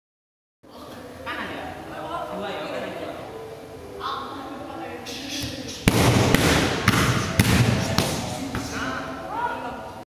Latihan speed fuul
didalam pencak silat banyak hal yg mempengaruhi kemenangan, salah satunya tekhnik, ayuk latihan tekhnik